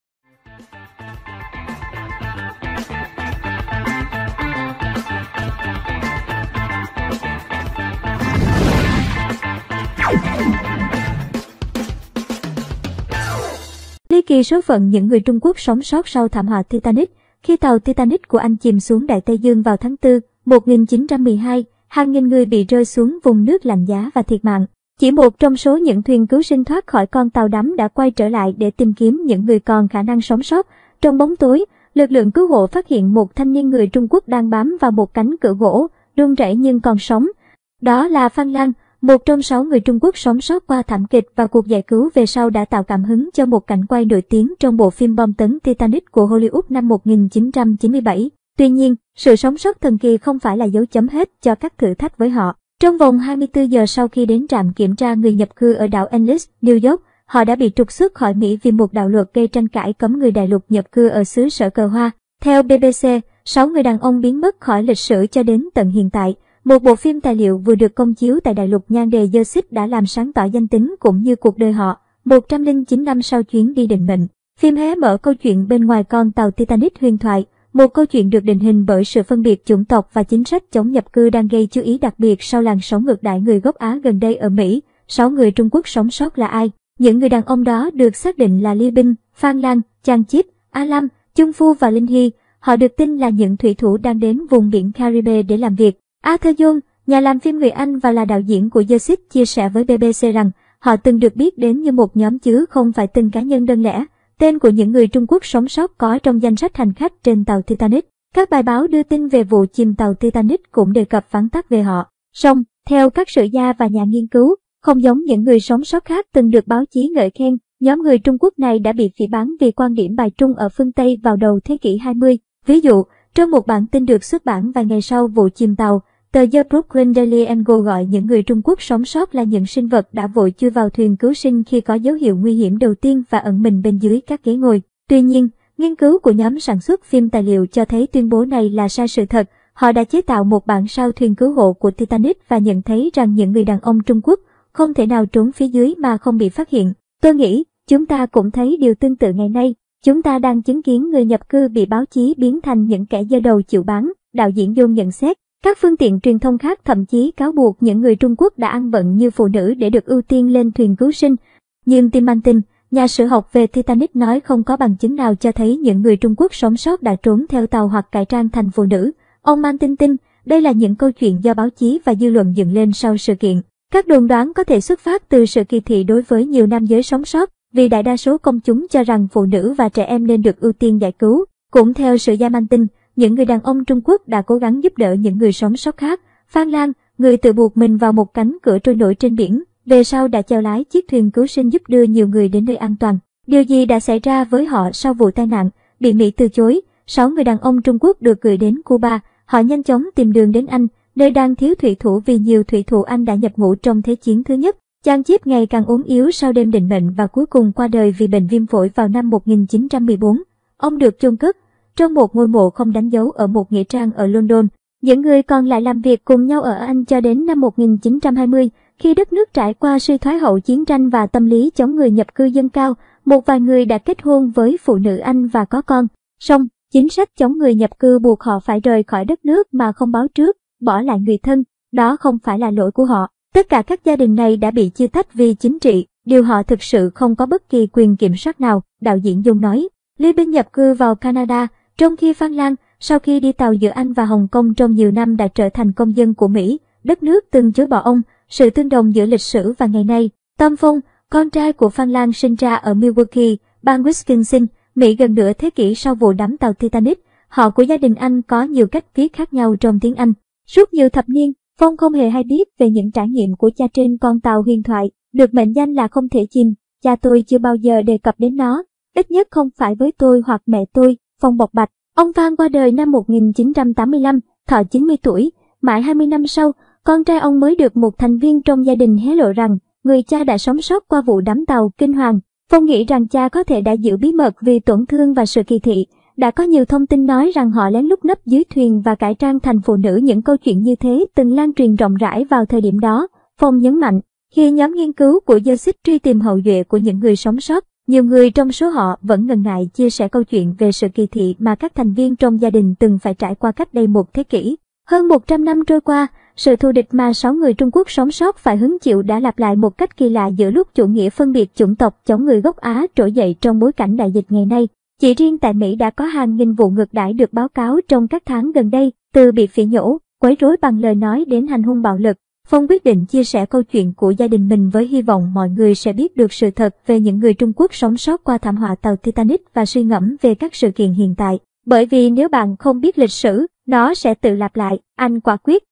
LY KỲ 6 NGƯỜI TRUNG QUỐC SỐNG SÓT TRONG VỤ CHÌM TÀU TITANIC - KIẾN THỨC ONLINE
- Khi tàu Titanic của Anh chìm xuống Đại Tây Dương vào tháng 4/1912, hàng nghìn người bị rơi xuống vùng nước lạnh giá và thiệt mạng.
- Chỉ một trong số những thuyền cứu sinh thoát khỏi con tàu đắm đã quay trở lại để tìm kiếm những người còn khả năng sống sót.